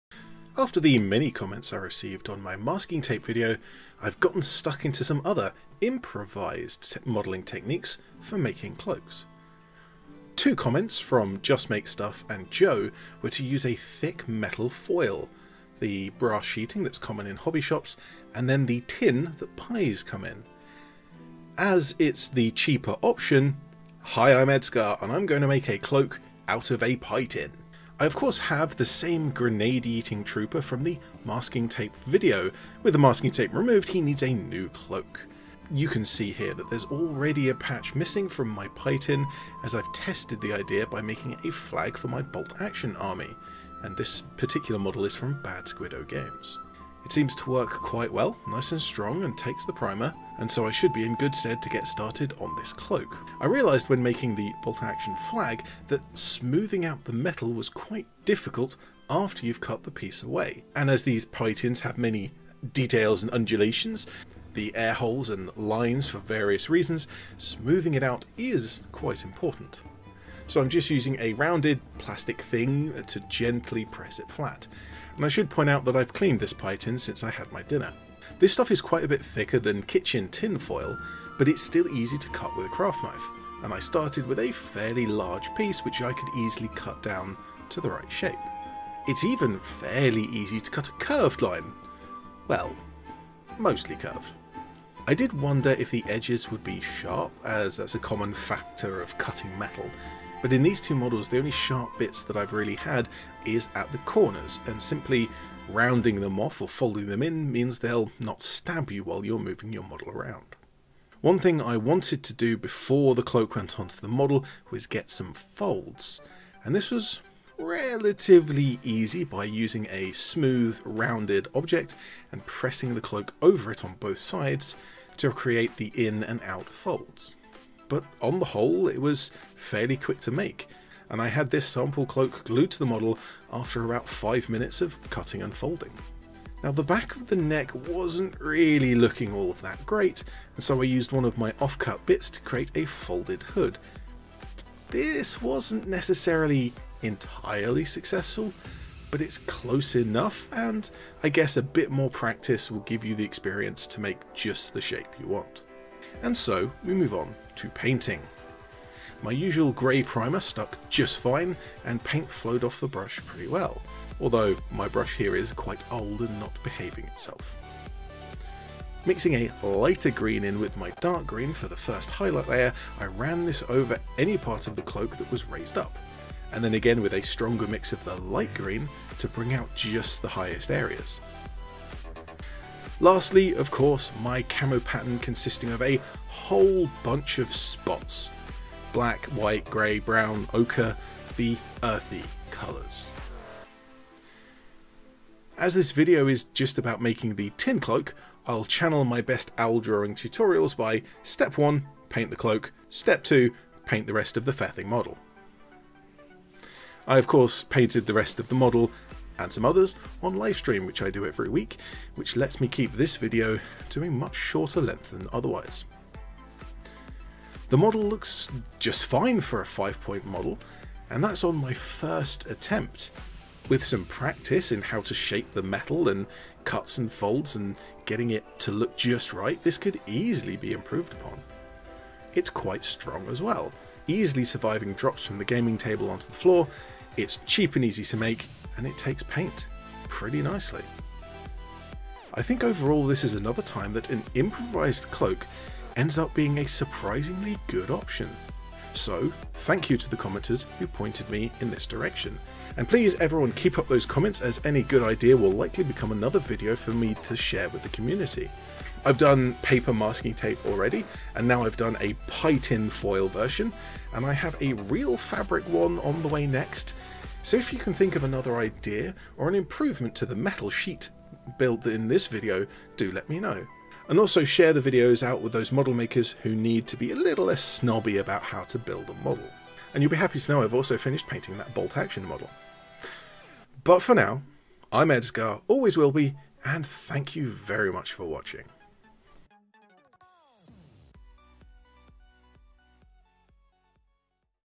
Improvised Cloaks: Modelling with a pie tin!
Second in my sub-series on making model cloaks for my Gaunt's Ghosts models is the thin sheet metal that you get from pie tins, a very cheap, and potentially quite strong material so long as it looks good when painted, will look great on your warhammer table.

Making these videos is a hobby for me, but if you wish to help me cover the costs of making them, you can donate, with my thanks,